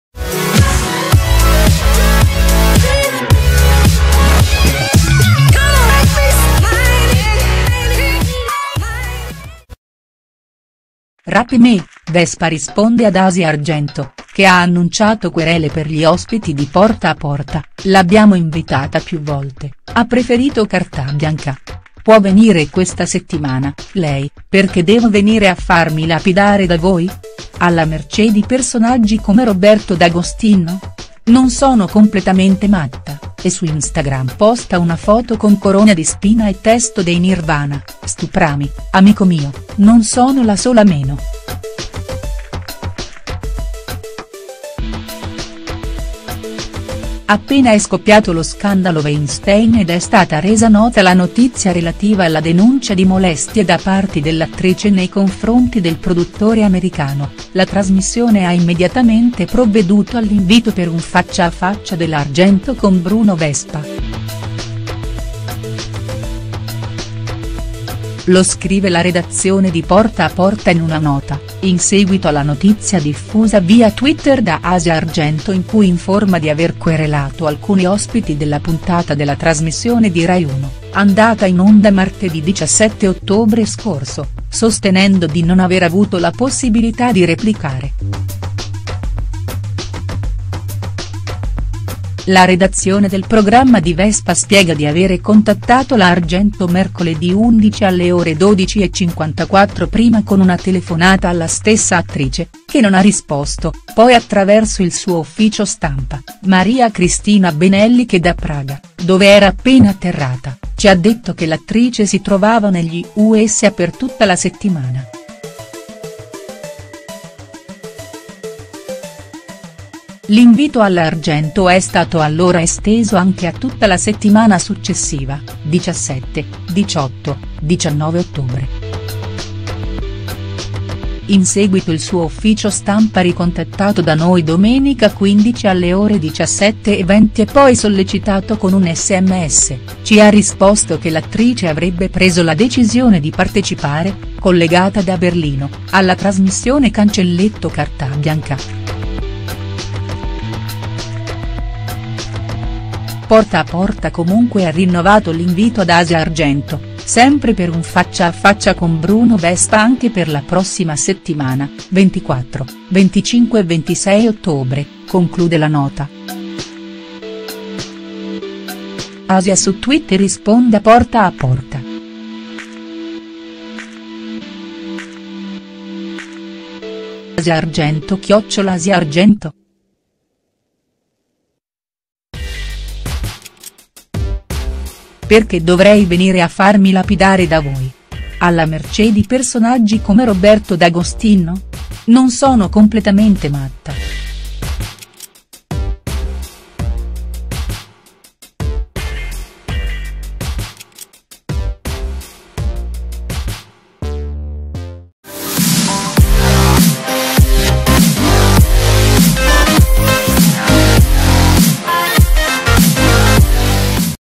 ''RAPE ME'' - VESPA RISPONDE AD ASIA ARGENTO, CHE HA ANNUNCIATO QUERELE PER GLI...-| STARS NEWS
Adnkronos ASIA ARGENTO INSTAGRAM RAPE ME   "Appena è scoppiato lo scandalo Weinstein ed è stata resa nota la notizia relativa alla denuncia di molestie da parte dell'attrice nei confronti del produttore americano, la trasmissione ha immediatamente